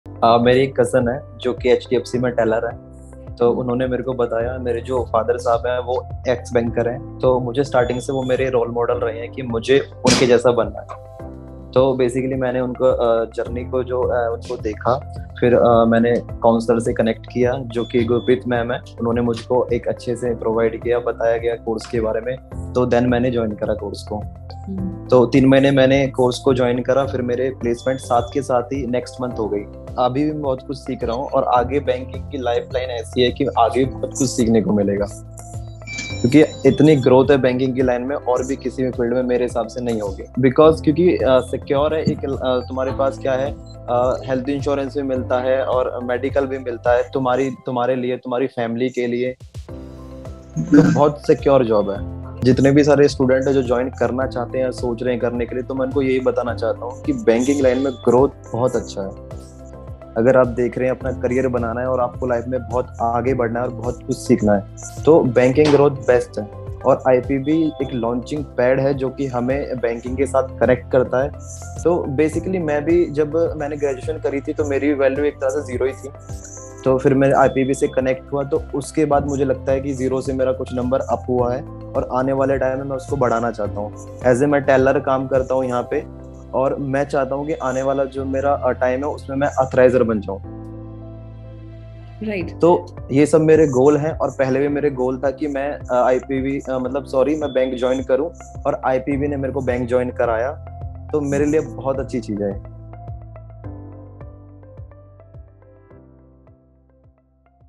Candidate Talk | Ajay Kumar | Institute of Professional Banking
Join us on the exhilarating journey of Candidate Talk IPB . Hear from our Student as he share their inspiring story.

We congratulate Ajay for his achievements and wish him the best for his future endeavors!

You can get this opportunity too. Join us today by registering for our PGCRB Course!